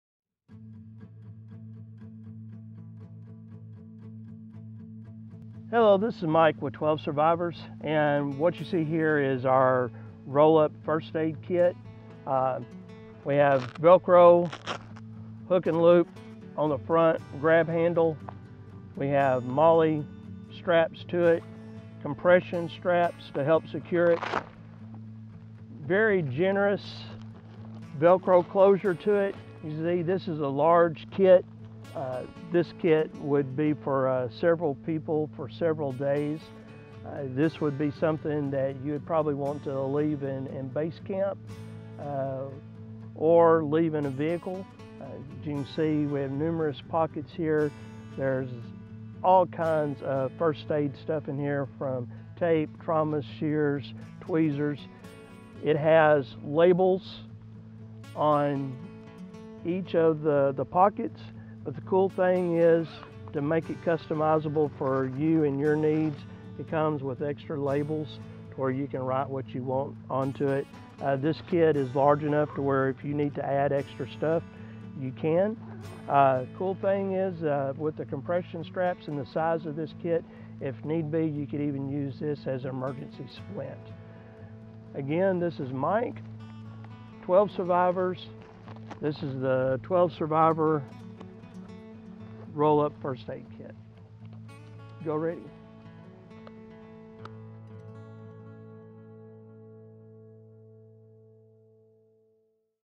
12 Survivors First Aid Rollup Kit - TS42000B
The 12 Survivors First Aid Rollup Kit offers the basics in life saving medical intervention. Its contents will treat injuries ranging from the basic knee scrape to the life threatening extremity hemorrhage and offer you the ability to provide the intervention necessary to save a life. The kit unrolls to reveal zipped pouches for organized storage and do-it-yourself labels so you can easily identify all of your supplies under critical stress. With snap button straps, the kit easily attaches to the exterior of existing gear, freeing up space in your pack and offering you unimpeded access to your medical supplies. Regardless of your environment, the 12 Survivors First Aid Rollup Kit provides the tools necessary to survive until help can be reached.

Key Features Include:
Zipped pouches for organized storage
Do-it yourself velcro labels
Easily attaches to the exterior of your gear
Weather resistant nylon
42-piece kit
Rollup case can be used as a make shift splint